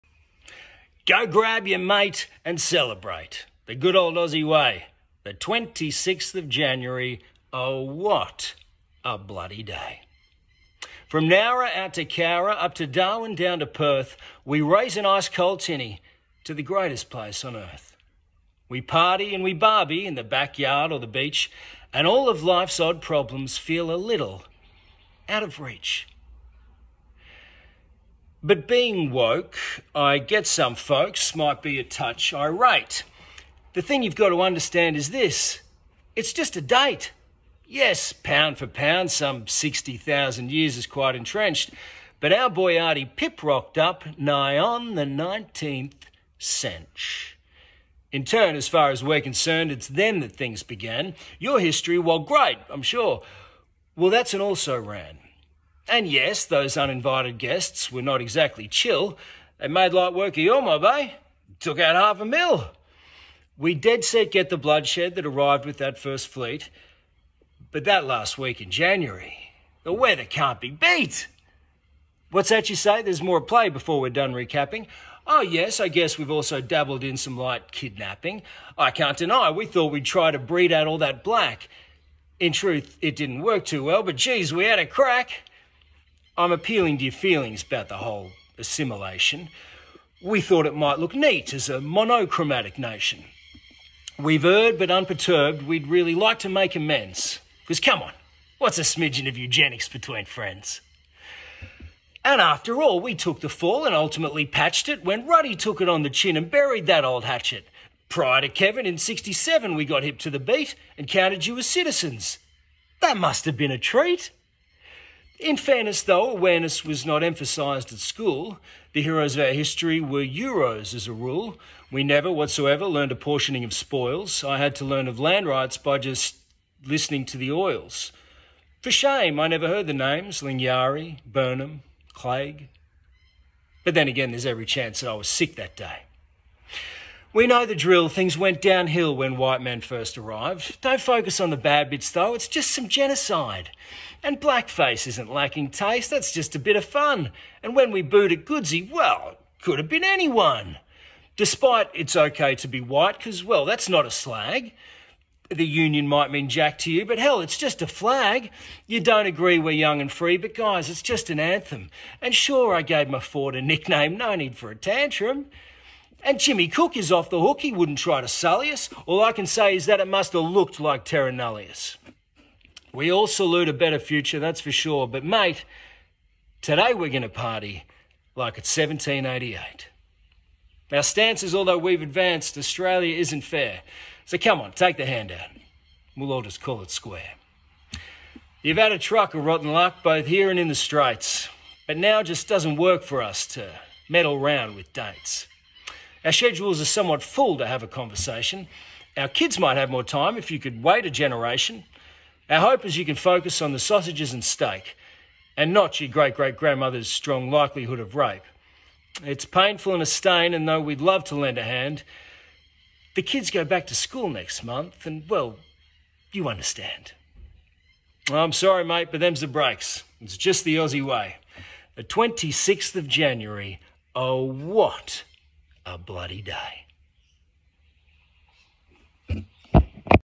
The 26th of January
Australia Day poem by Ben Lawson